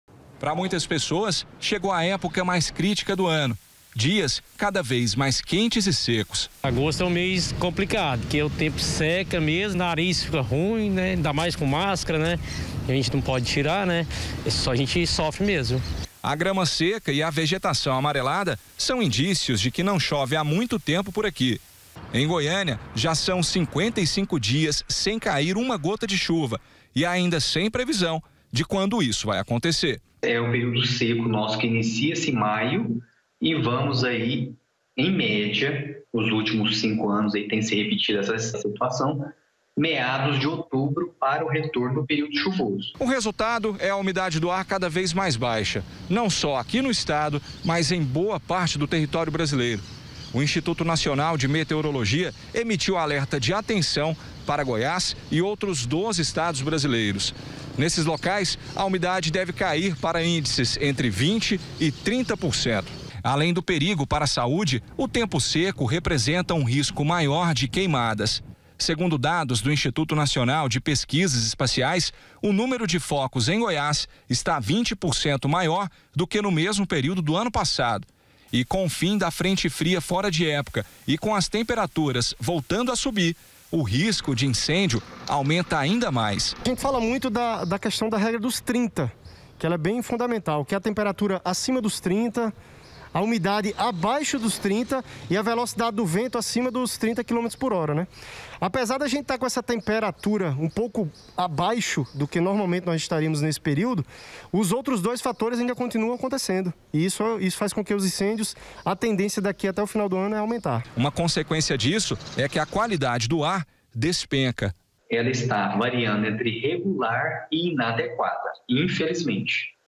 UMIDADE DO AR: INMET EMITE ALERTA DE ATENÇÃO PARA GOIÁS
NÃO CHOVE EM GOIÂNIA HÁ 55 DIAS E A UMIDADE DO AR ESTÁ CADA VEZ MENOR, O QUE É UM RISCO PRA SAÚDE, PRINCIPALMENTE DE CRIANÇAS E IDOSOS. O INMET EMITIU UM ALERTA PRO ESTADO.